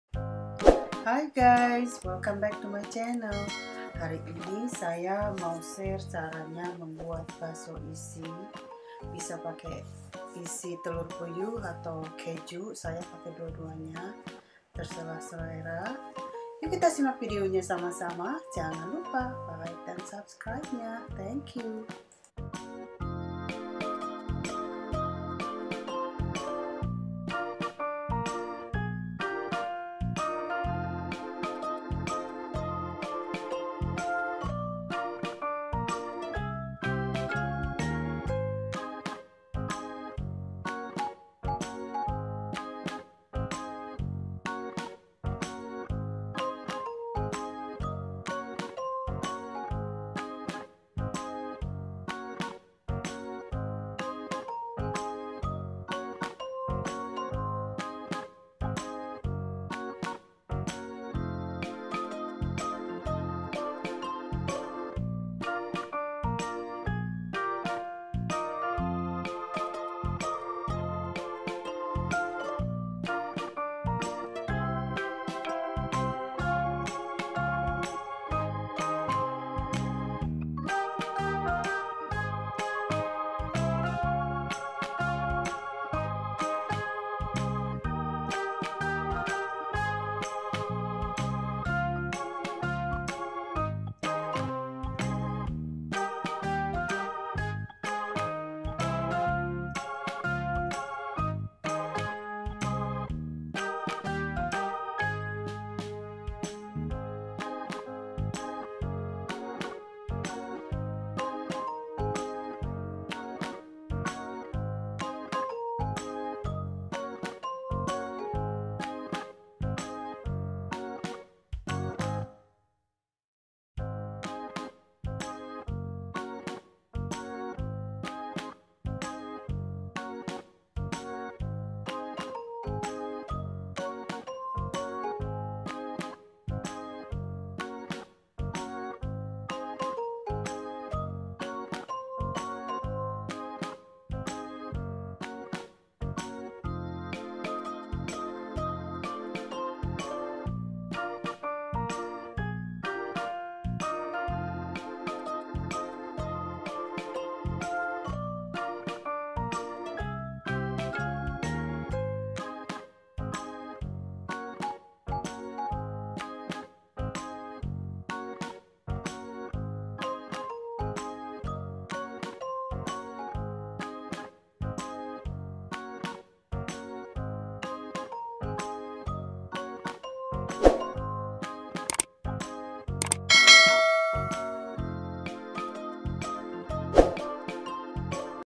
Bakso Isi Telur Puyuh || Bakso Isi Keju
Bahan -Bahan
1 kg daging sapi giling
5 putih telur
1 cangkir es batu
12 sdm tepung tapioka
1 sdm goreng bawang merah
1 sdm goreng bawang putih
1 sdt baking soda
1 sdt baking powder
1/2 sdt lada bubuk
1 sdm kaldu rasa sapi
1 sdt garam
Bahan isi
telur puyuh secukup nya
keju mozarela secukup nya